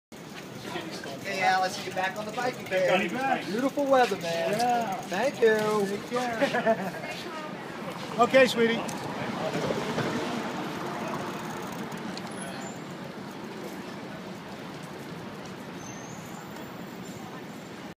Al Roker on Bicycle
Al roker commutes on 2 wheels.